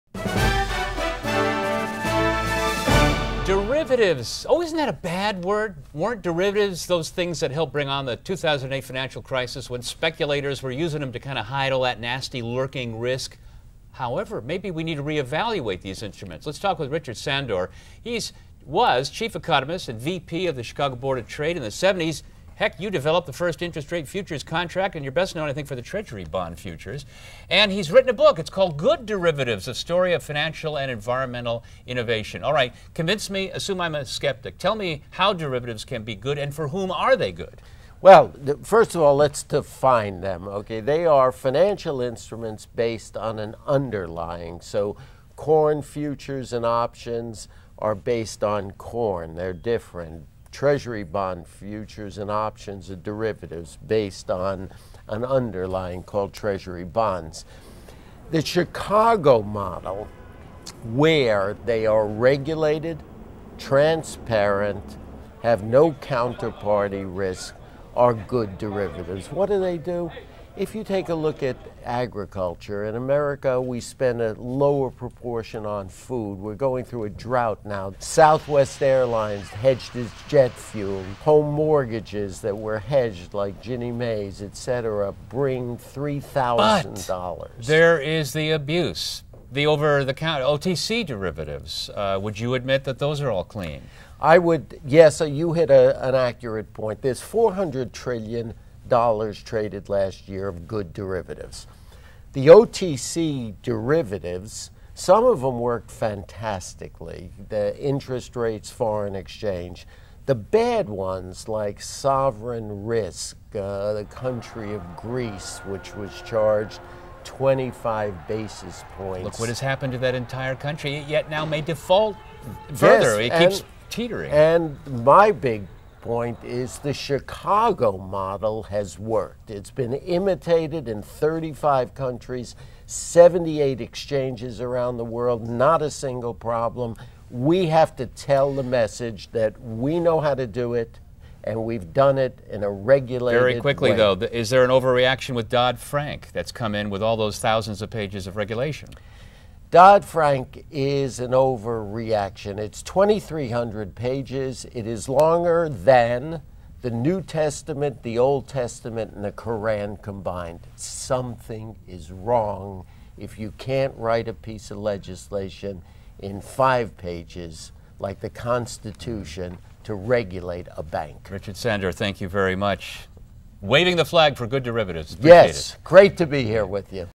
Good Derivatives 8-24-12
Bill Moller talks derivatives with Richard Sandor, former chairman of the Chicago Board of Trade.